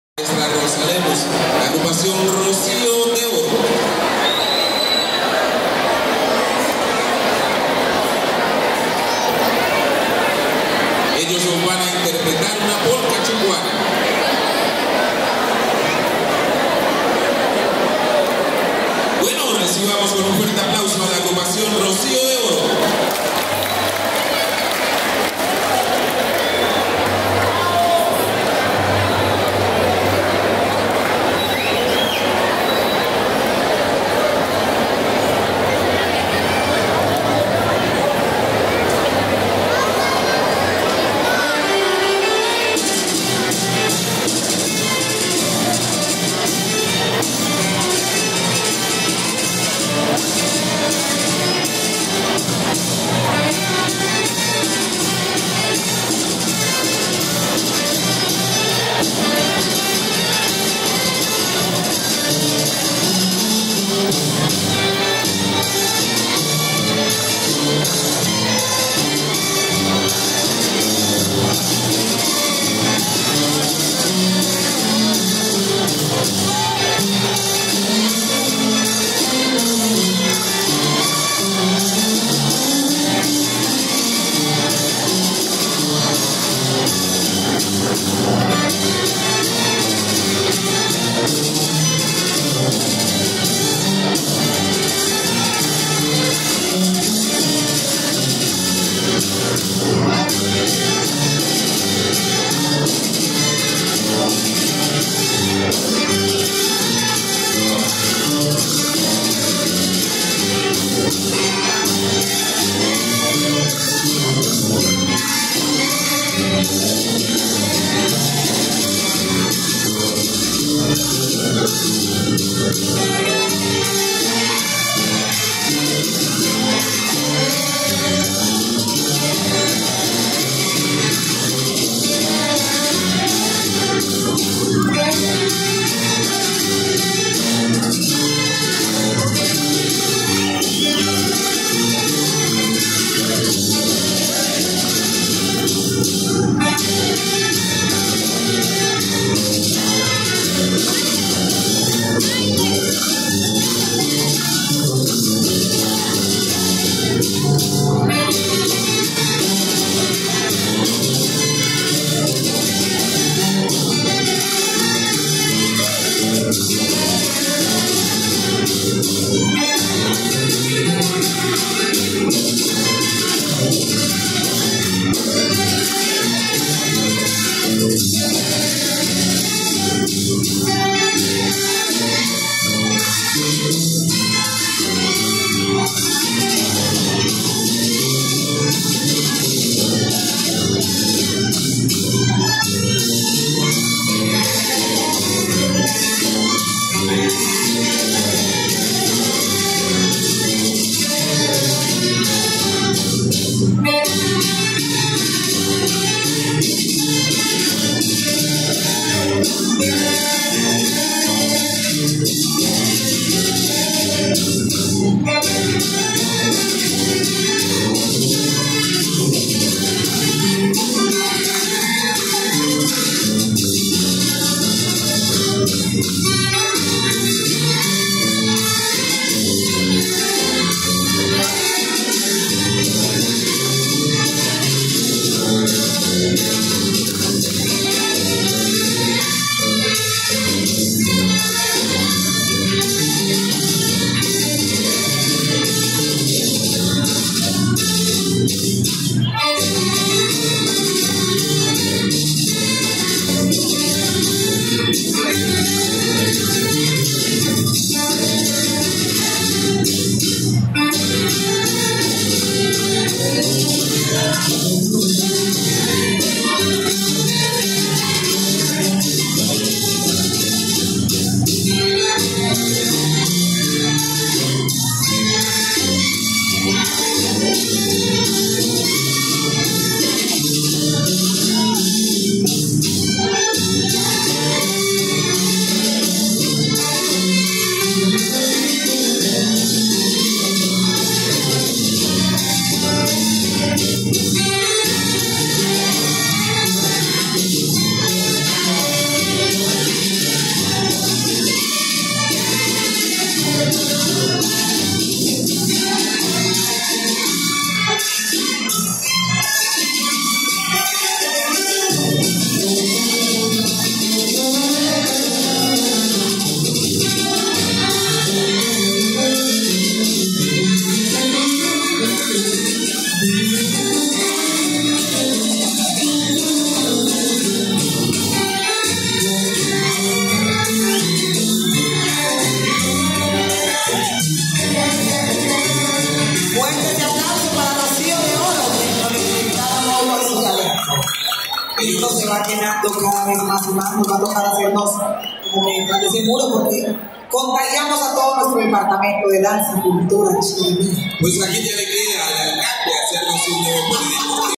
Grupo de Danza Rocío de Oro - Jotaton Tadó 2024
Una Polca Chocoana es el ritmo musical elegido por el Grupo de Danza Chocoana Rocío de Oro para la presentación en el municipio San José de Tadó en el Departamento del Chocó.

En el marco de este evento de masiva participación, vale la pena destacar la próxima realización del Festival Petronio Álvarez 2024 que se llevará a cabo del 14 al 19 de agosto en la ciudad de Cali.

El Festival de Música del Pacífico Petronio Álvarez nació en 1997 y recibió su nombre en honor al músico bonaverense Patricio Romano Petronio Álvarez Quintero, un talentoso poeta y compositor que desde muy pequeño aprendió a tocar la guitarra y cuyas canciones narraban la cotidianidad de su territorio y rápidamente formaron parte de la cultura afrocolombiana.

A pesar de que tuvo que trabajar como notario para subsistir, Petronio formó en 1935, a sus 21 años de edad, el conjunto musical “Buenaventura”, junto con quienes compuso múltiples bambucos, merengues, sones, huapangos y abozaos.

Durante los primeros años del Festival Petronio Álvarez se celebraba una sola modalidad en la que se mezclaban distintos ritmos y expresiones de la música del Pacífico colombiano, pero en la actualidad existen cuatro modalidades, cada una con un enfoque particular.

Las primeras categorías en introducirse fueron la modalidad Conjunto de Chirimía, que está ligada a la tradición musical del departamento del Chocó; la categoría Conjunto de Marimba dedicada a este instrumento que caracteriza al Pacífico Sur del país.

Posteriormente se crearon las modalidades Versión Libre que relaciona la música urbana moderna con las composiciones tradicionales del territorio.

La última categoría, Violines Caucanos, fue introducida en el 2008 y le dio un lugar especial a la charanga caucana, una corriente musical protagonizada por instrumentos de viento y percusión que es muy famosa en el Pacífico.

Los ganadores de cada una de las categorías que componen al festival harán vibrar a Bogotá con la magia ancestral del Pacífico colombiano y darán inicio a una de las fiestas más importantes de todo el país que celebra la afrocolombianidad desde una de las expresiones más importantes que tiene: la música.